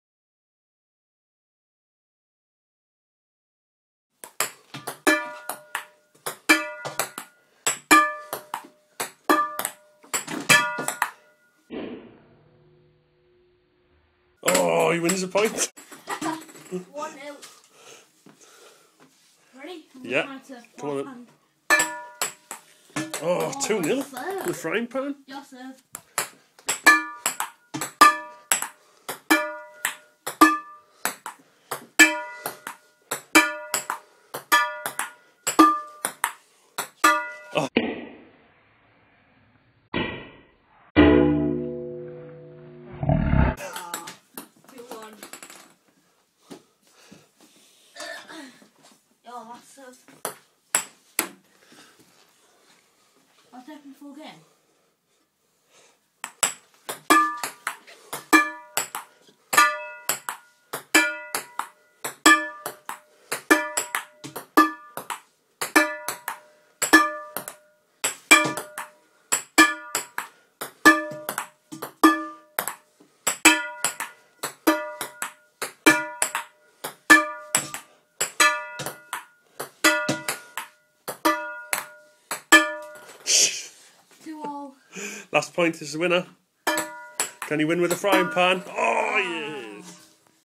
frying pan ping pong
frying pan ping pong with slo mo action replay